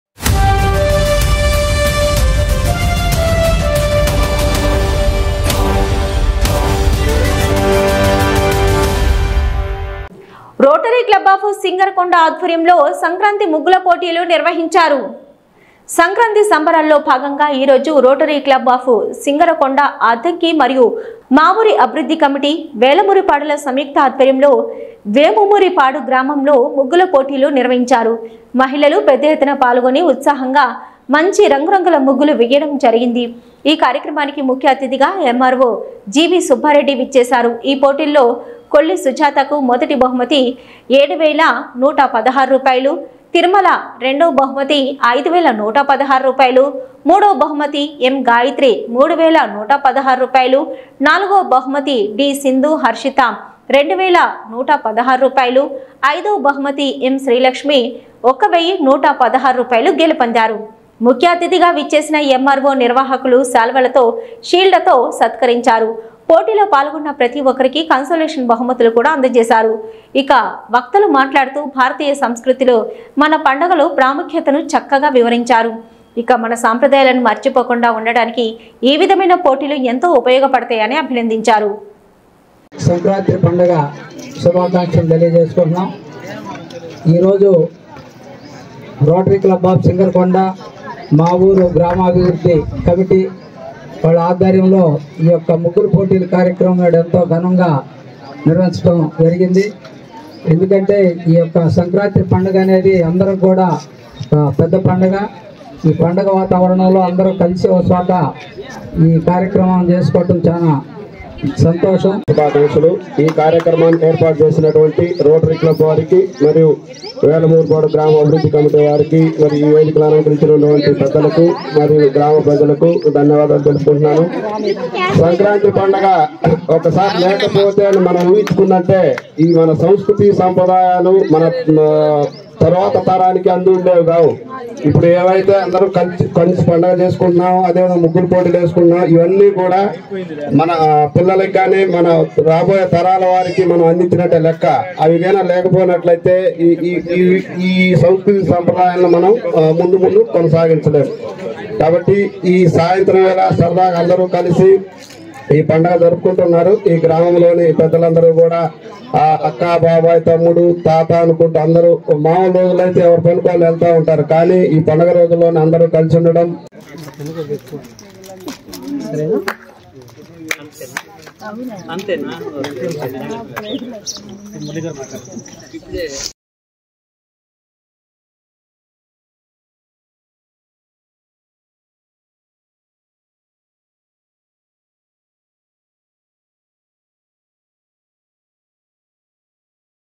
సంక్రాంతి ముగ్గులుపోటీలు ....రోటరీ క్లబ్ ఆఫ్ సింగర్ కొండ#bapatladistrict
రోటరీ క్లబ్ ఆఫ్ సింగర్ కొండ ఆధ్వర్యంలో సంక్రాంతి ముగ్గులుపోటీలు నిర్వహించారు
సంక్రాంతి సంబరాల్లో భాగంగా ఈరోజు రోటరీ క్లబ్ ఆఫ్ సింగరకొండ అద్దంకి మరియు మా ఊరు అభివృద్ధి కమిటీ వేలమూరిపాడుల సంయుక్త ఆధ్వర్యంలో , వేలమూరిపాడు గ్రామంలో ముగ్గుల పోటీలు నిర్వహించారు . మహిళలు పెద్ద ఎత్తున పాల్గొని ఉత్సాహంగా మంచి రంగు రంగుల ముగ్గులు వేయడం జరిగింది. ఈ కార్యక్రమానికి ముఖ్యఅతిథిగా ఎమ్మార్వో జీవి సుబ్బారెడ్డి . విచ్చేశారు. ఈ పోటీల్లో కొల్లి సుజాతకు

 మొదటి బహుమతి రు 7116/-. వి తిరుమల రెండవ బహుమతి రు 5116/-. మూడో బహుమతి ఎం గాయత్రి రు 3116/-. నాలుగవ బహుమతి డి. సింధు హర్షిత రు 2116. ఐదవ బహుమతి ఎం శ్రీలక్ష్మి రు1116 గెలుపొందారు . , ముఖ్య అతిధిగా విచ్చేసిన ఎమ్మార్వో.నిర్వాహకులు శాలువాలతో , షీల్డ్ లతో సత్కరించారు పోటీలో పాల్గొన్న ప్రతి ఒక్కరికి కన్సోలేషన్ బహుమతులు కూడా అందజేశారు.
వక్తలు మాట్లాడుతూ. భారతీయ సంస్కృతిలో మన పండుగల ప్రాముఖ్యతను చక్కగా వివరించారు. మన సంప్రదాయాలను మర్చిపోకుండా ఉండటానికి ఈ విధమైన పోటీలు ఎంతో ఉపయోగపడతాయని అభినందించారు .  చిన్న చిన్నగ్రామంలో ఈ పోటీలు జరగడం సంతోషదాయకమన్నారు ఈ కార్యక్రమంలో రోటరీ అధ్యక్షులు చిన్ని మురళీకృష్ణ , కార్యదర్శి చప్పిడి వీరయ్య , కోశాధికారి కొల్లా భువనేశ్వరి , ఏజి లెవి ప్రసాద్ , సందిరెడ్డి శ్రీనివాసరావు , కాకర్ల వెంకటేశ్వర్లు , ఊటుకూరి  ఉమాఅన్నపూర్ణ , మా ఊరు అభివృద్ధి కమిటీ సభ్యులు ఉప్పుటూరి అచ్యుత్  మరియు వారి మిత్ర బృందం వేలమూరిపాడువాసులు తదితరులు పెద్ద సంఖ్యలో పాల్గొన్నారు .